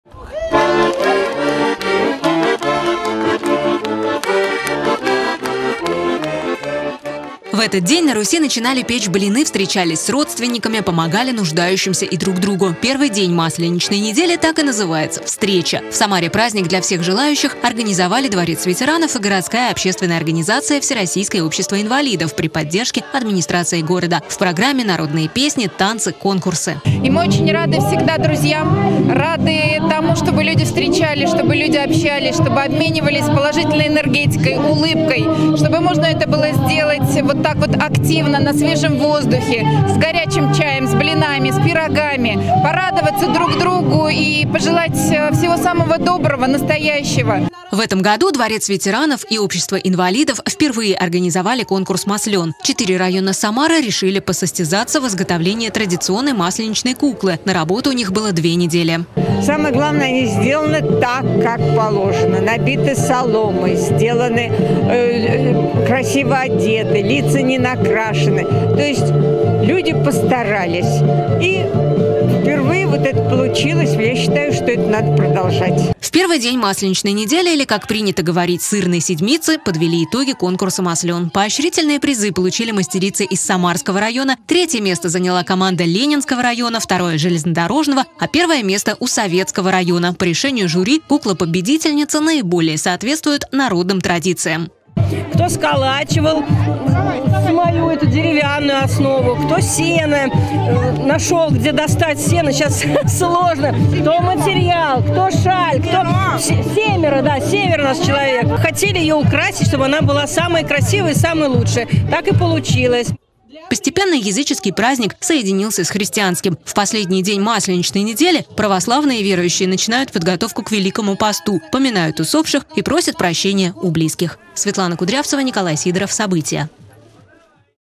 В Самаре отметили первый день масленичной недели
В России началась Масленица - праздник, сохранившийся с языческих времён. Из года в год дата праздника меняется. Это связано с особенностями Пасхи. Каждый год её отмечают в разные числа. В этом году она приходится на 16 апреля. Соответственно, Великий пост начинается за 47 дней, 27 февраля. Поэтому в этом году празднование Масленицы начинается в понедельник 20 февраля, а завершается 26 числа Прощеным воскресеньем. В Самаре первый день масленицы отметили народными гуляньями. На них побывала наша съемочная группа.